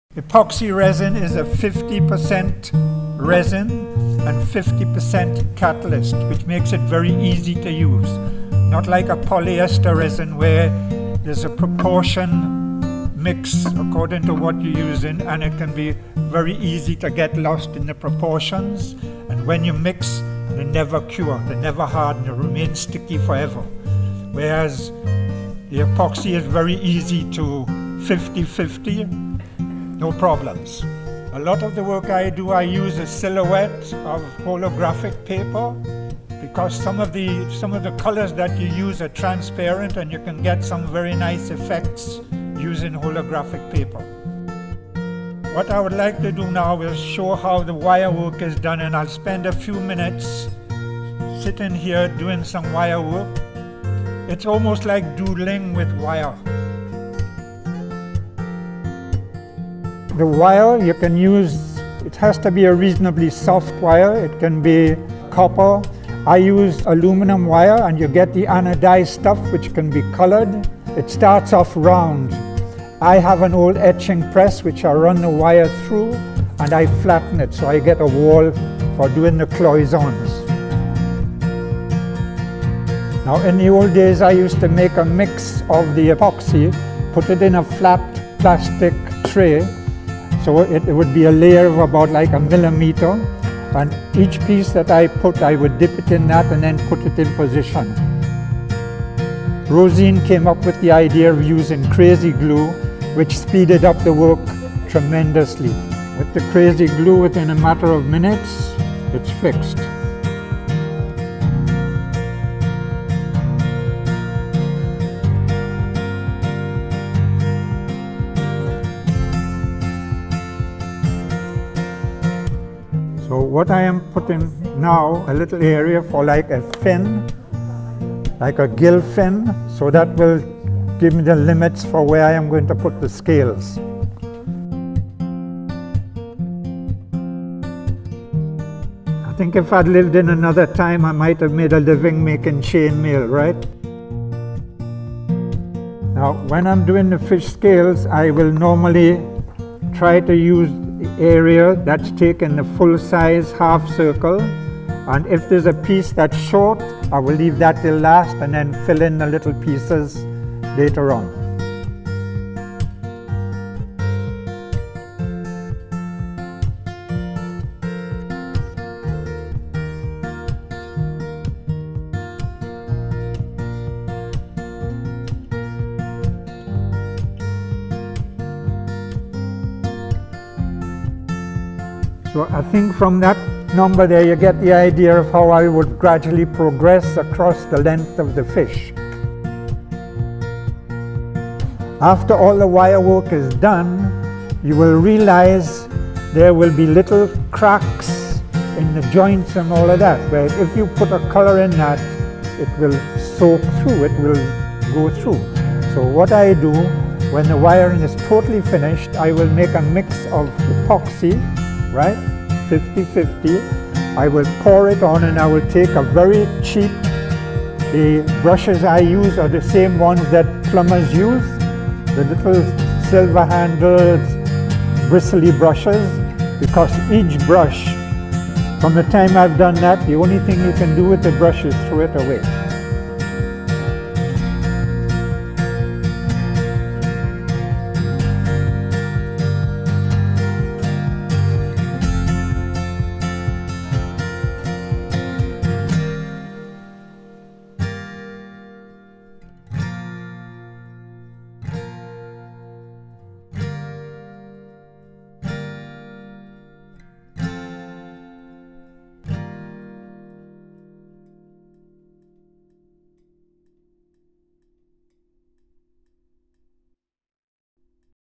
Cloisonné demonstration with Robert Mackie
Demonstration from the Cypress Art League, March general meeting.

Robert E. Mackie, was born in Sunderland, England. As a young teenager, he left the bleak climate of Northeast, England for the warmth of the Caribbean. He spent one year in Jamaica, before settling in Trinidad, the southernmost island in the chain. Living on the North Coast of the island, with its lush mountains, verdant valleys, green bamboo forests and meandering rivers, it was a move to “Paradise.” It was here that Bob’s love for the Caribbean landscape and his palette of tropical hues first emerged.

Bob attended both Fatima and St. Mary’s Colleges in the 1950’s before training as a textile engraver with Textile Mills, Arima. Bob maintained a professional career as a production manager for several decades, working for local and international companies but his most memorable work experience involved a sojourn as a manager of SIMLA, a scientist research station, in the remote Arima valley. Since SIMLA was affiliated with the Bronx Zoo, New York, Bob encountered many naturalists and scientists working in Trinidad and had the opportunity to accompany them on expeditions deep into Trinidad’s rainforests, swamps and caves. Once again, his knowledge of, and love for, the landscape and the flora and fauna of the island, was cultivated.

Art-wise, Bob is totally self taught, a “chameleon” of styles and techniques that he has developed and mastered over years of experimentation. He has produced works in metal sculpture, watercolor, acrylic and oil painting, paper collage and cold enameling (cloisonné). His natural artistic talent was inherited from his father, a man he never knew. Bob’s father, Robert H. Mackie, died as a volunteer in the Spanish Civil War, just months before the birth of his son. Bob’s prize possessions are two watercolors, painted by his father.

Bob first exhibited his work at Trinidad Art Society’s (TAS) Independence Exhibition in 1962. He has held annual one-man shows in Trinidad for the past 40 years and has also exhibited in numerous group shows in the Caribbean, California and London. Bob’s artwork continues to be in demand in Trinidad, where it adorns the walls of private residences and established businesses. He is a regular exhibitor at Horizon’s Art Gallery in Port of Spain, Trinidad.

Bob presently resides in Redondo Beach, California. He designs for the Tile Guild Inc. in Los Angeles, specializing in Renaissance tiles, as well as one-of-a-kind murals and back splashes for homes in the exclusive communities of Malibu and Beverly Hills. His work has appeared in Architectural Digest and other national publications.